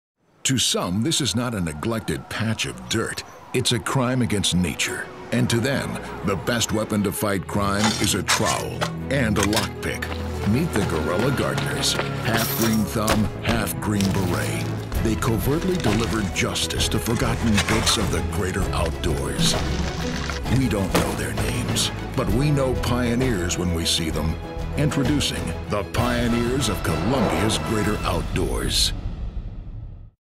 Guerrilla Gardeners
Meet the Guerilla Gardeners, part of Columbia Sportswears new brand campaign featuring the Pioneers of the Greater Outdoors.